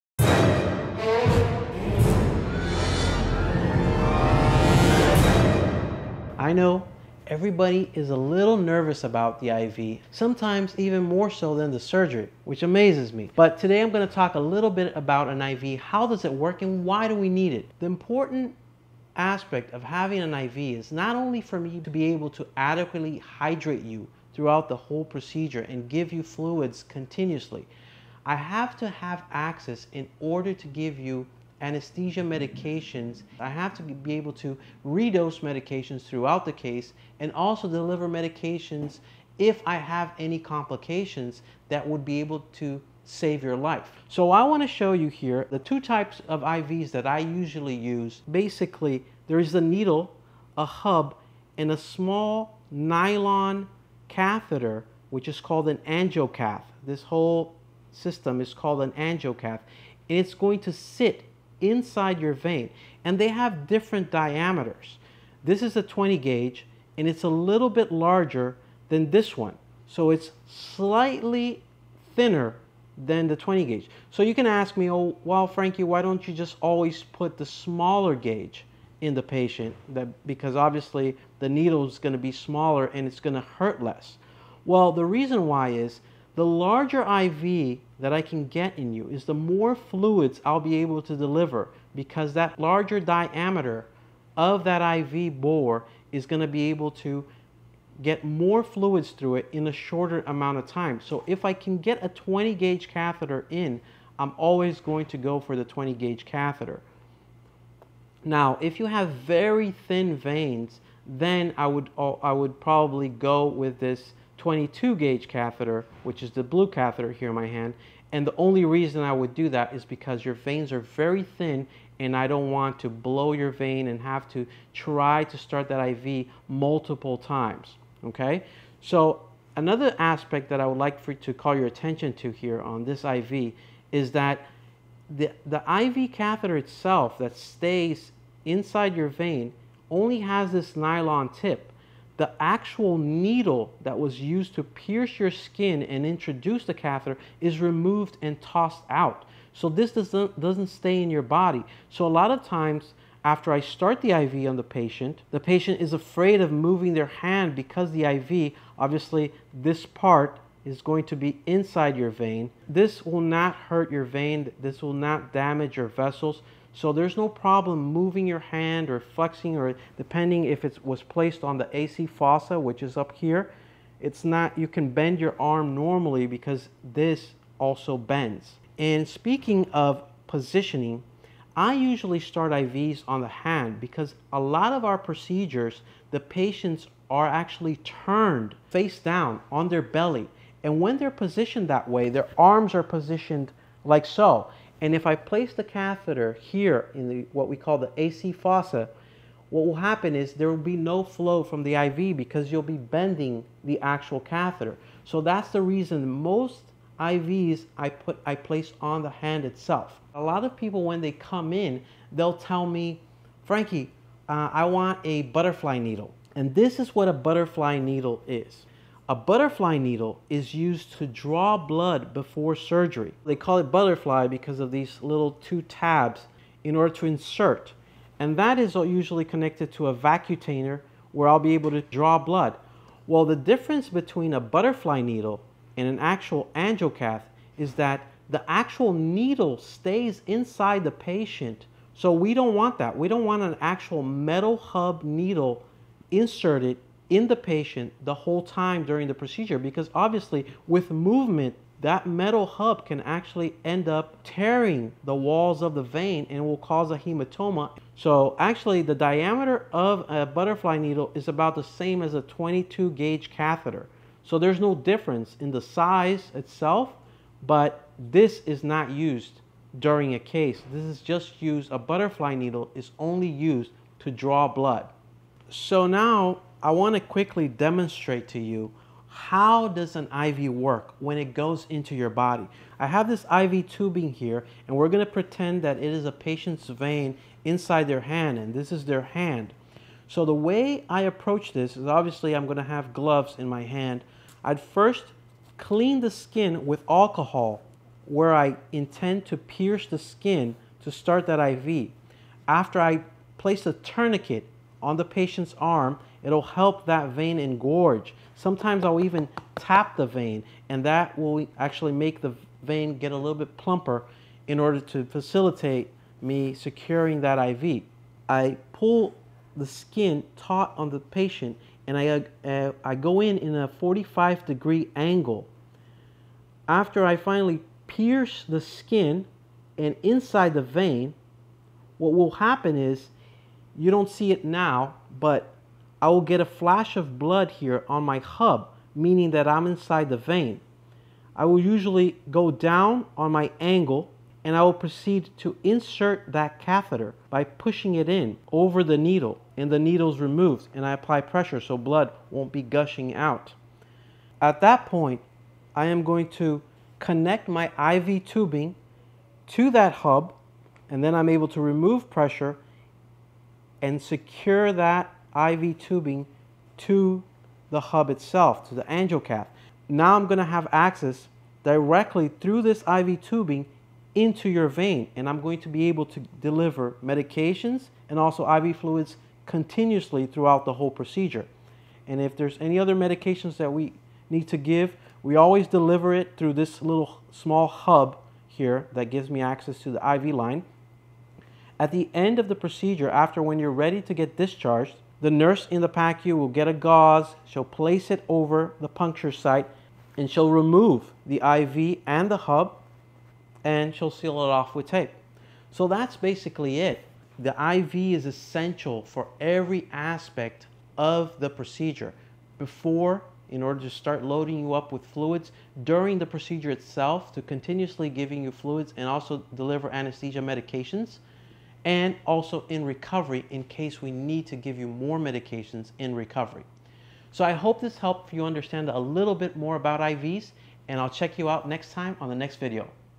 IV for Surgery: Why do you need it? How to start an IV.
Why do we need an IV catheter for surgery? And how do you start one? In this video, I explain the entire process, from choosing a vein to inserting the catheter and securing it in place. I also discuss the importance of IV catheters in surgery and how they help to keep patients safe and comfortable.

Here's what you'll learn in this video:

Why IV catheters are necessary for surgery
How to prepare the IV site
How to insert the IV catheter
How to secure the IV catheter in place
Whether you're a medical professional or simply curious about how IV catheters work, this video is for you. I've explained the process in a clear and concise way, with step-by-step instructions and helpful tips.

Watch now and learn how to start an IV catheter like a pro!